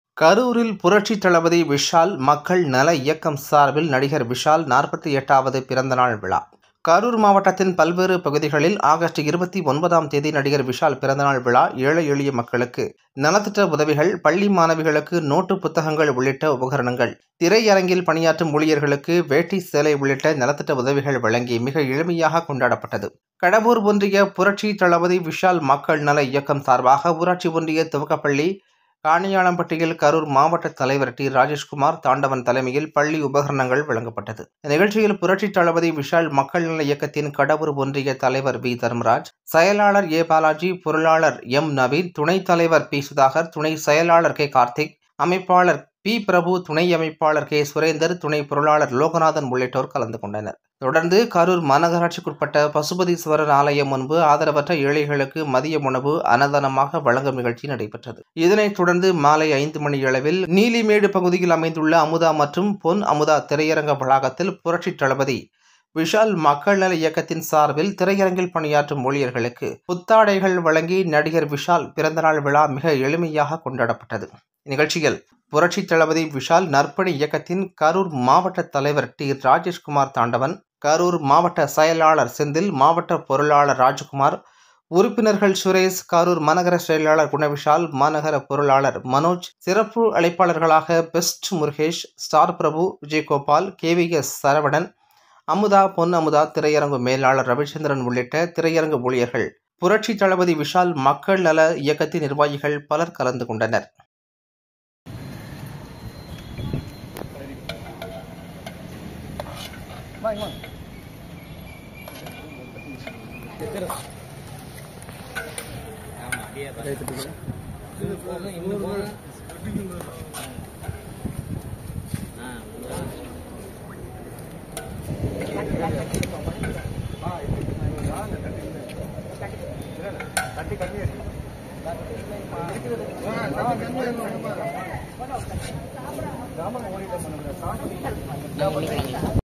கரூரில் புரட்சி தளபதி விஷால் பிறந்தநாள் விழா
கரூரில் புரட்சி தளபதி விஷால் மக்கள் நல இயக்கம் சார்பில் நடிகர் விஷால் 48 வது பிறந்தநாள் விழா...

கரூர் மாவட்டத்தின் பல்வேறு பகுதிகளில் ஆகஸ்ட் 29ஆம் தேதி நடிகர் விஷால் பிறந்தநாள் விழா ஏழை எளிய மக்களுக்கு நலத்திட்ட உதவிகள், பள்ளி மாணவிகளுக்கு நோட்டு புத்தகங்கள் உள்ளிட்ட உபகரணங்கள், திரையரங்கில் பணியாற்றும் ஊழியர்களுக்கு, வேட்டி சேலை உள்ளிட்ட நலத்திட்ட உதவிகள் வழங்கி மிக எளிமையாக கொண்டாடப்பட்டது.

 கடவூர் ஒன்றிய புரட்சிதளபதி விஷால் மக்கள் நல இயக்கம்
சார்பாக , ஊராட்சி ஒன்றிய துவக்கப்பள்ளி காணியாளம்பட்டியில் ,
கரூர் மாவட்ட தலைவர் T.ராஜேஷ்குமார் தாண்டவன் தலைமையில்,
பள்ளி மாணவர்களுக்காக படிப்பு உபகரணங்கள்
வழங்கப்பட்டது

இந்நிகழ்ச்சியில், புரட்சித் தளபதி விஷால் மக்கள் நல இயக்கத்தின் கடவூர் ஒன்றிய தலைவர் V. தர்மராஜ்  , செயலாளர்
A.பாலாஜி ,  பொருளாளர்
M. நவீன் ,துணைத்தலைவர்
P. சுதாகர் ,துணைச்செயலாளர்
K.கார்த்திக் ,அமைப்பாளர்
P.பிரபு ,துனை அமைப்பாளர்
K.சுரேந்தர் ,துனைபொருளாளர்
லோகநாதன் உள்ளிட்டவர்கள் கலந்து கொண்டனர்.

தொடர்ந்து கரூர் மாநகராட்சிக்கு உட்பட்ட பசுபதீஸ்வரர் ஆலயம் முன்பு ஆதரவற்ற ஏழைகளுக்கு மதிய உணவு அன்னதானமாக வழங்கும் நிகழ்ச்சி நடைபெற்றது.

இதனைத் தொடர்ந்து , மாலை 5 மணியளவில், கரூர் நீலிமேடு பகுதியில் அமைந்துள்ள அமுதா மற்றும் பொன் அமுதா திரையரங்க வளாகத்தில் புரட்சித்தளபதி விஷால் மக்கள் நல இயக்கத்தின் சார்பில் திரையரங்கில் பணியாற்றும் ஊழியர்களுக்கு, புத்தாடைகள் வழங்கி நடிகர் விஷால் பிறந்தநாள் விழா மிக எளிமையாக கொண்டாடப்பட்டது.

இந்நிகழ்ச்சியில், புரட்சித்தளபதி விஷால் நற்பணி இயக்கத்தின் கரூர் மாவட்ட தலைவர் T.ராஜேஷ்குமார்தாண்டவன், கரூர் மாவட்ட செயலாளர் செந்தில், மாவட்ட பொருளாளர் ராஜ்குமார், உறுப்பினர்கள் சுரேஷ், கரூர் மாநகரச் செயலாளர் குணவிஷால், மாநகர பொருளாளர் மனோஜ்  அழைப்பாளர்களாக பெஸ்ட் முருகேஷ், ஸ்டார்பிரபு, விஜய்கோபால், KVSசரவணன், அமுதா பொன் அமுதா திரையரங்கு மேலாளர் ரவிச்சந்திரன் உள்ளிட்ட திரையரங்கு ஊழியர்கள் புரட்சி தளபதி விஷால் மக்கள் நல இயக்கத்தின் நிர்வாகிகள் கலந்து கொண்டனர்.